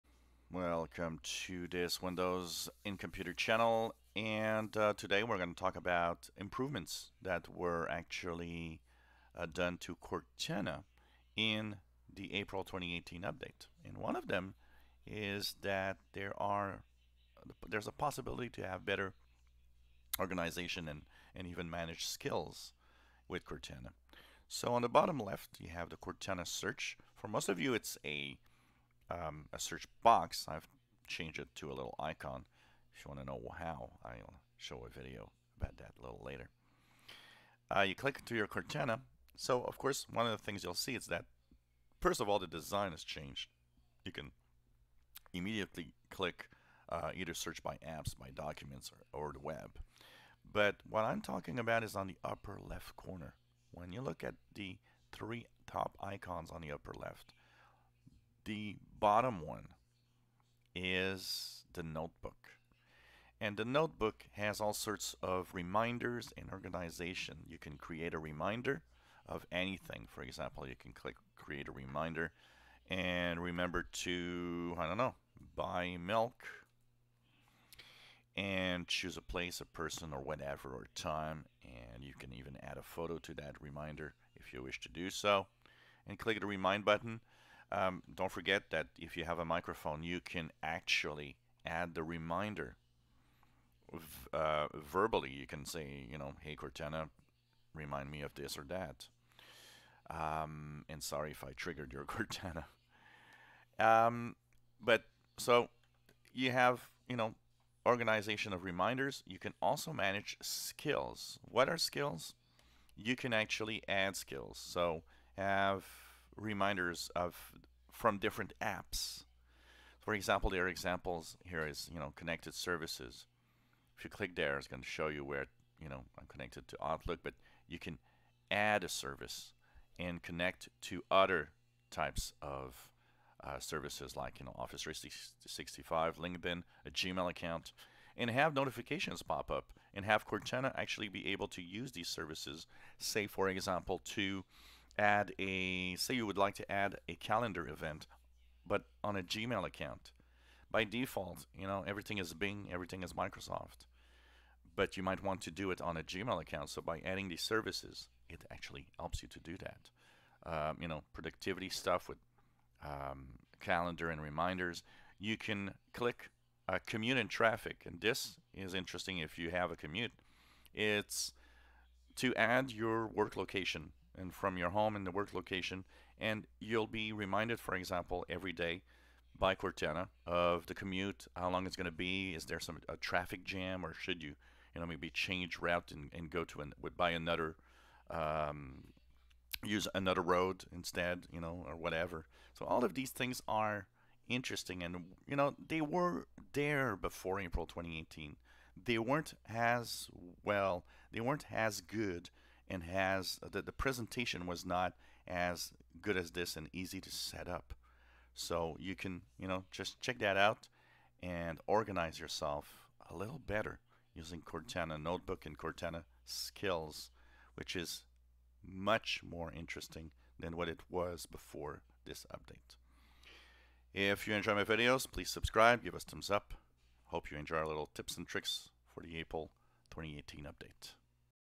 Windows 10 April 2018 update Improved Cortana Notebook skills and organization
Cortana has some improved skills in April 2018 update